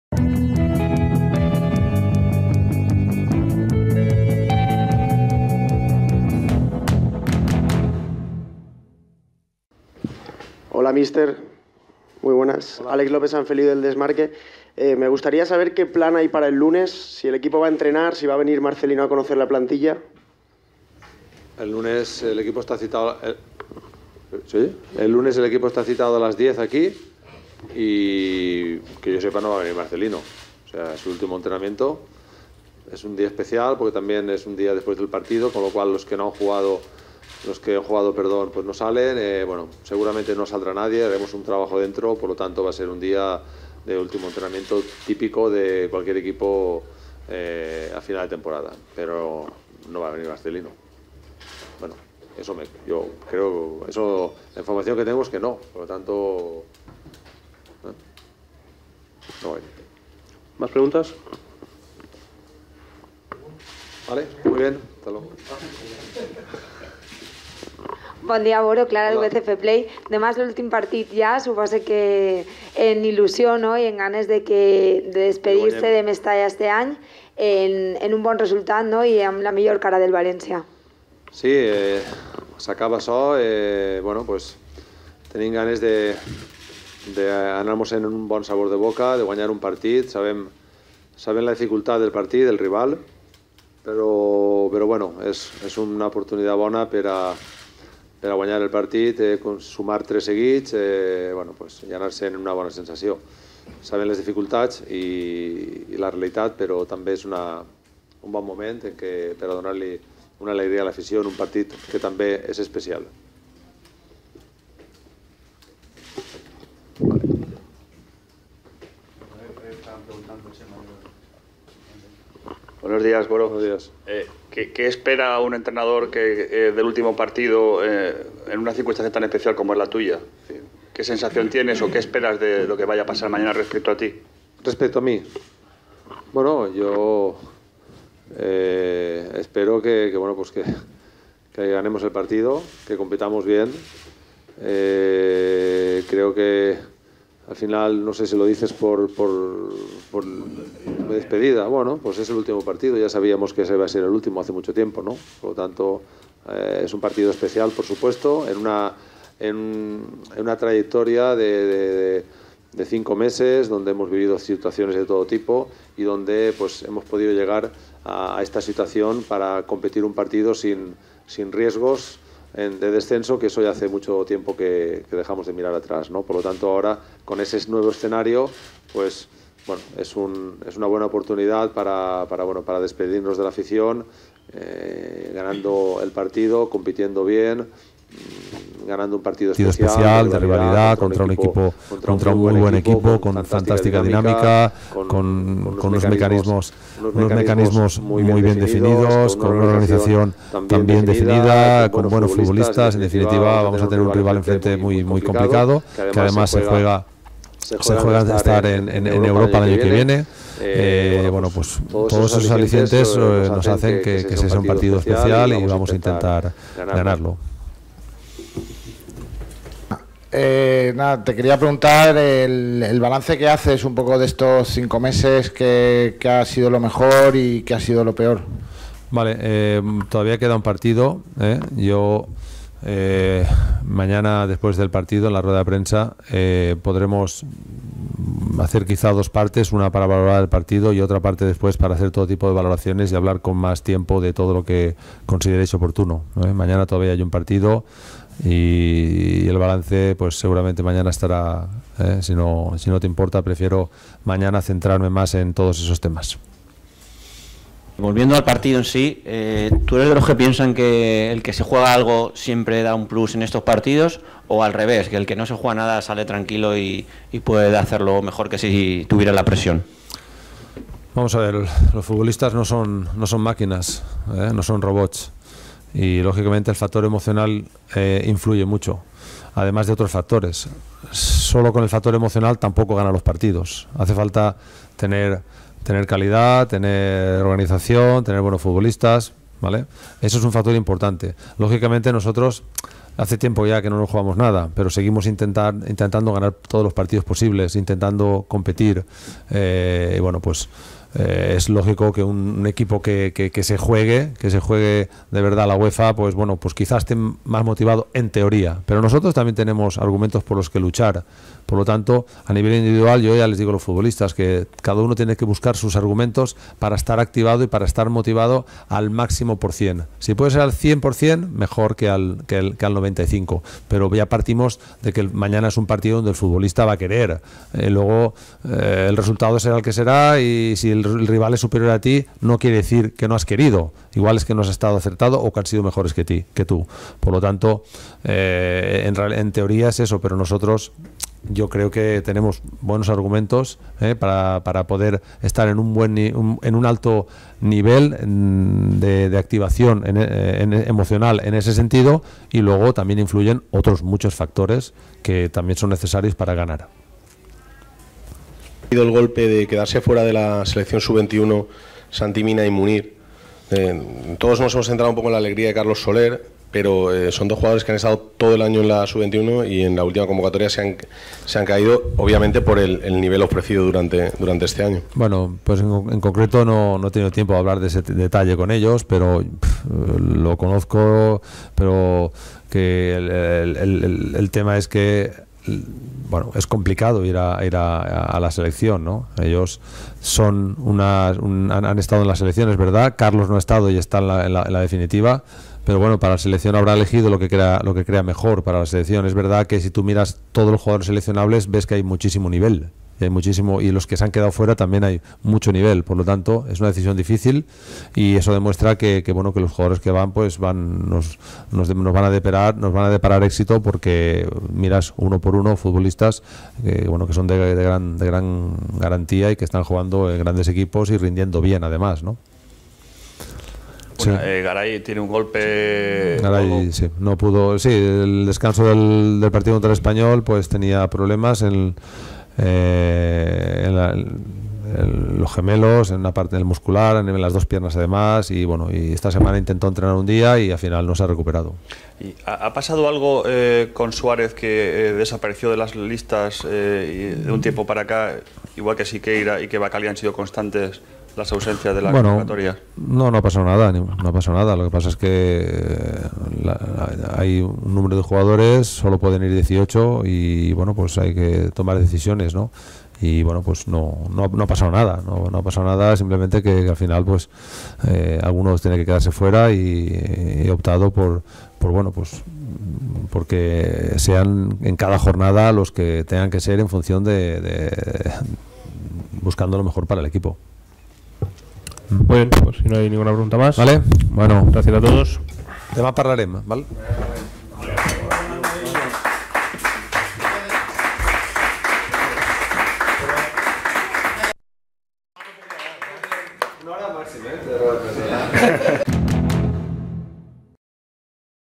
Rueda de prensa de Voro previa al Villarreal
El entrenador del Valencia CF, Voro, ha atendido a los medios de comunicación antes de que su equipo cierre la temporada 16/17 en Mestalla contra el Villarreal.